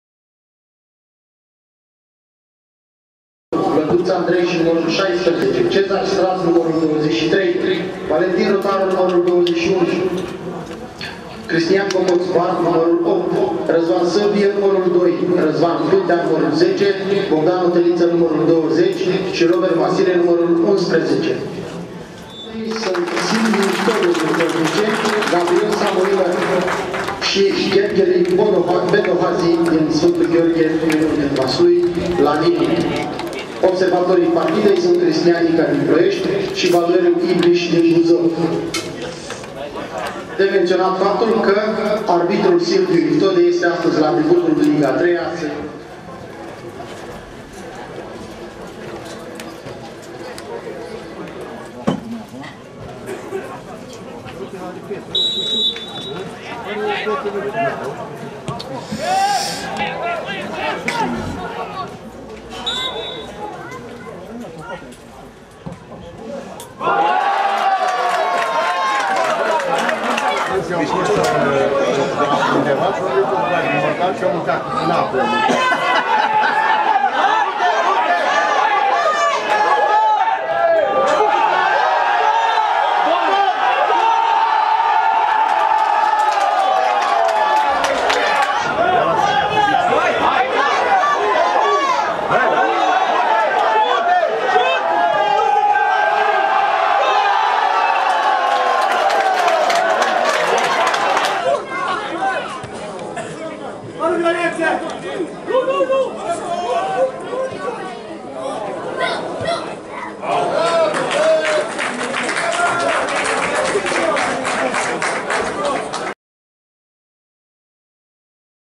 Etapa 19 | Ceahlaul Piatra Neamt - Stiinta Miroslava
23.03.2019, Stadionul Ceahlaul, Piatra Neamt

Rezumatul partidei dintre Ceahlaul Piatra Neamt si Stiinta Miroslava

Liga a III-a - seria I - sezon 2018/2019 - Etapa 19